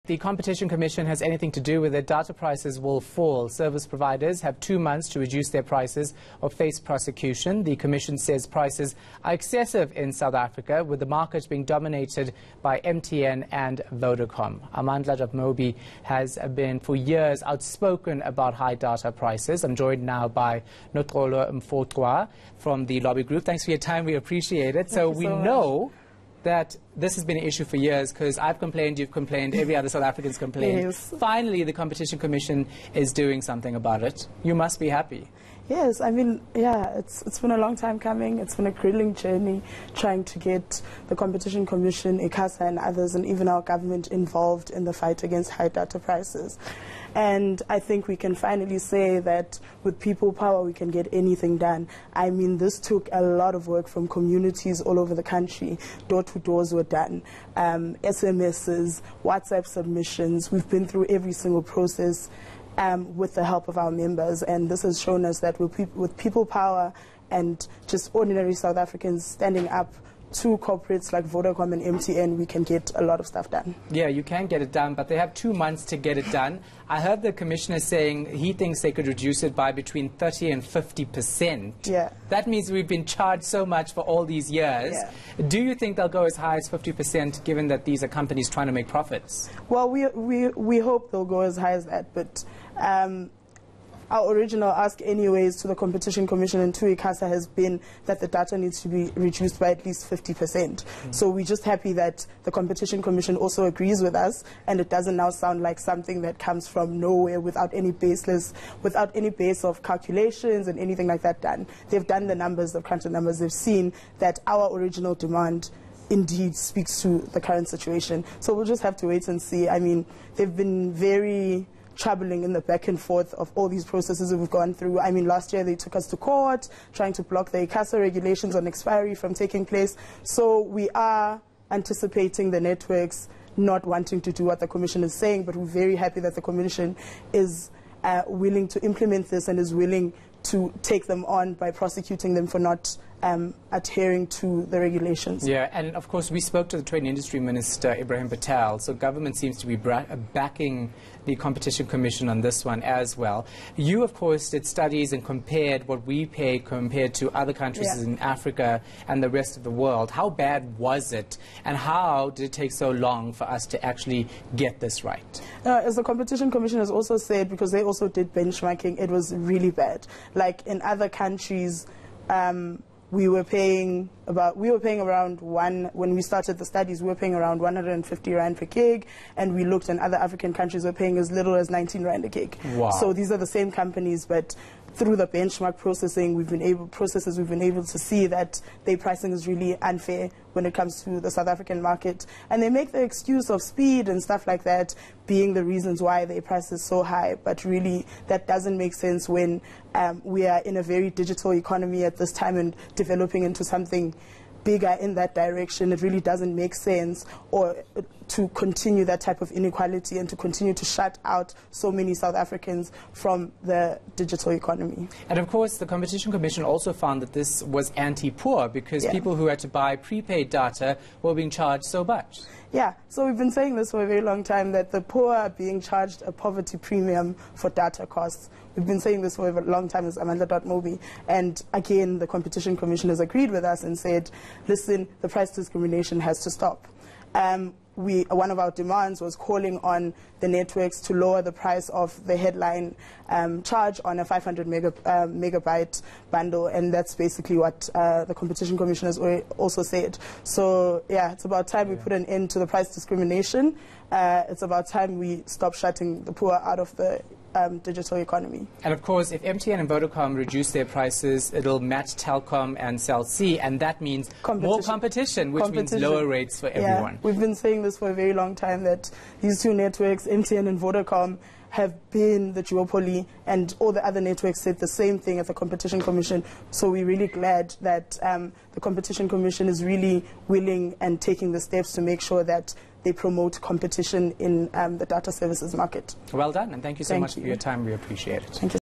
Amandla.mobi has for years been outspoken about high data prices
If the Competition Commission has anything to do with it, data prices will fall.
Service providers have two months to reduce their prices or face prosecution.
The Commission says prices are excessive in South Africa, with the market being dominated by MTN and Vodacom.
Courtesy